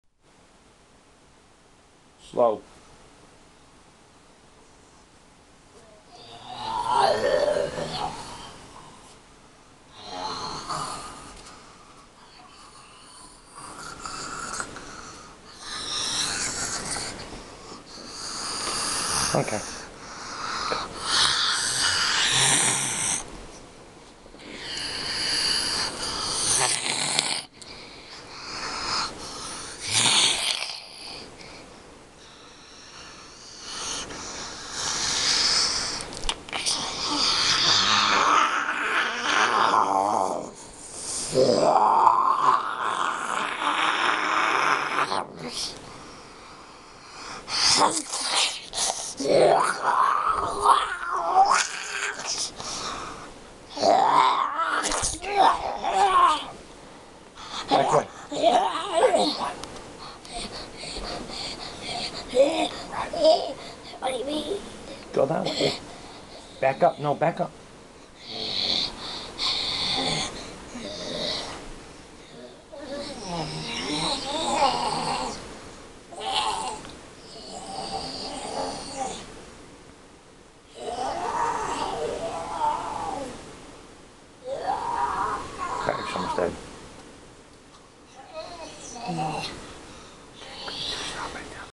Scary Bloody Zombie Filmed after Halloween!
Dan, who usually sings on here, has decided to try out for The Walking Dead, as a zombie. Sorry for the grainy video. Please be kind, and give Dan a thumbs up for his acting debut!!